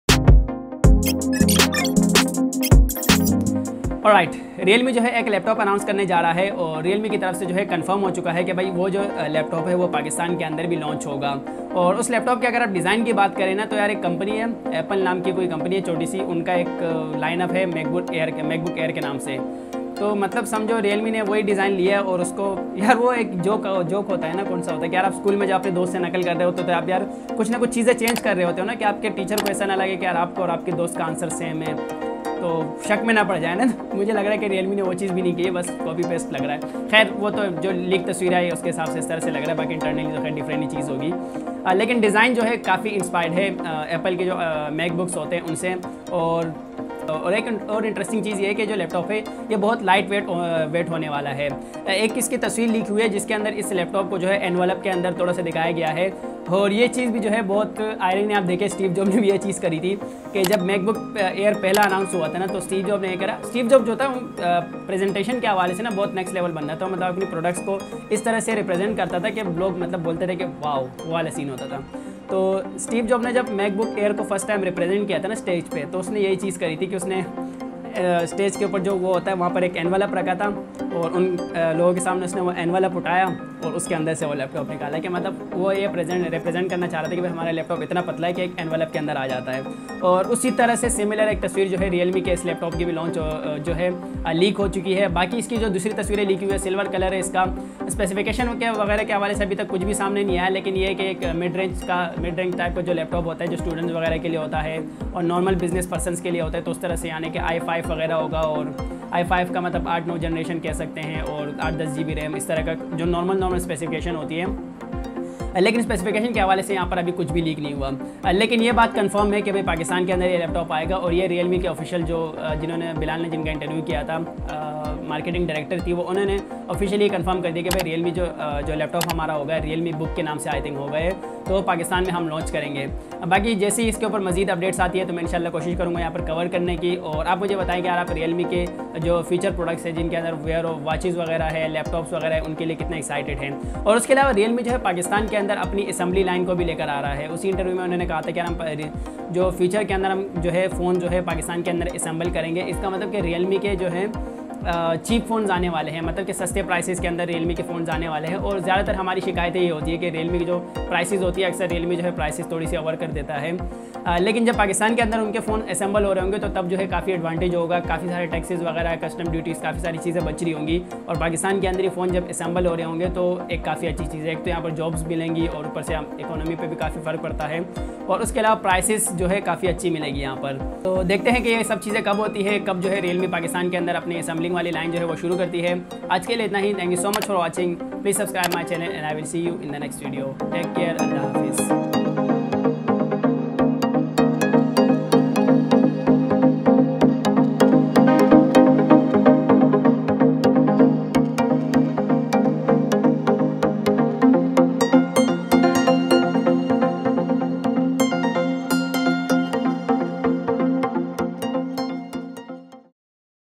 Realme Laptop Launching in Pakistan! Realme Book First Look
Realem is about to Launch Realme Laptop in Pakistan and its gona be called Realme Book. its been confirmed by Realme officials that Realme Laptop will be launching in Pakistan.